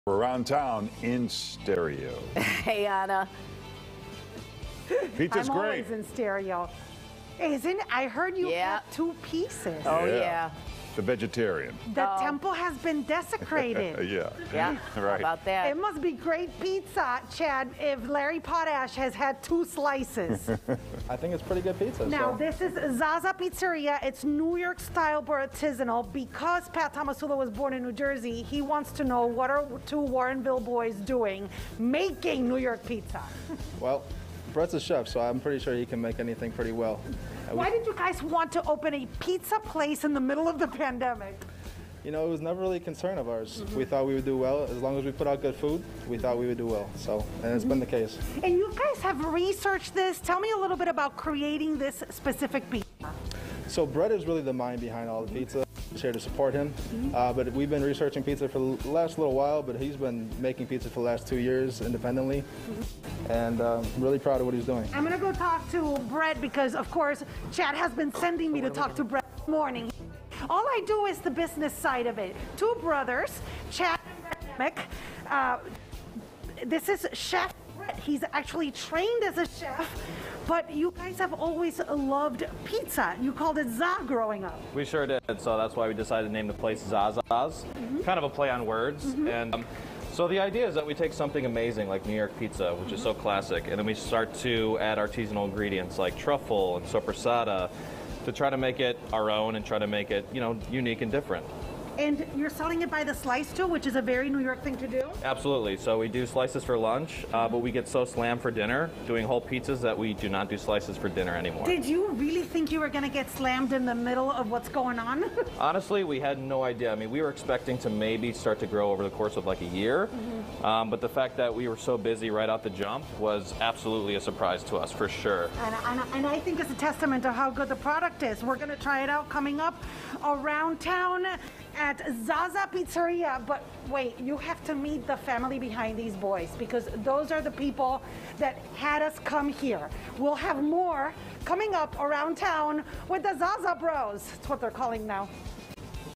Around Town - Zaza's Pizzeria
Ana speaks with Chadd and Chef Brett